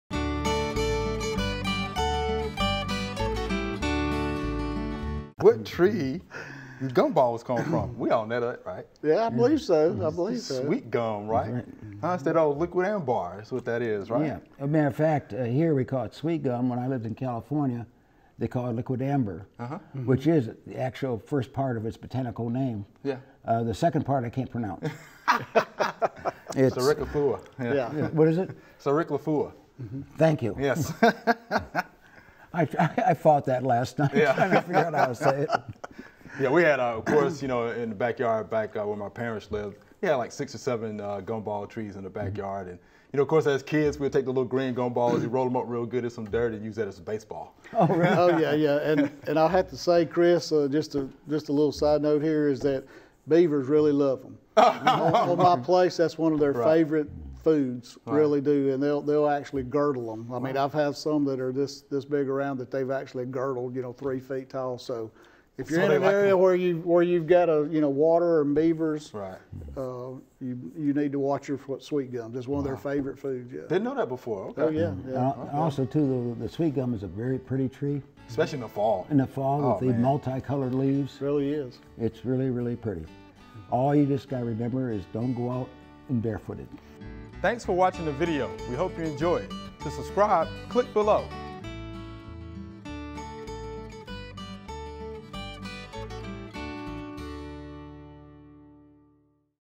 Q&A - What tree do gum balls come from?
Gum balls come from the sweetgum tree. They are called by many other names including liquid amber (The tree is in the Liquidambar genus). The gumballs are the seed pods. They start out green but turn brown and fall off the tree. Sweet gum trees have a nice shape and have beautiful fall foliage often with leaves that are several different colors.  Shelby County Extension Director Jim Todd does note that beavers like sweet gum trees and can girdle and kill them.

for more information!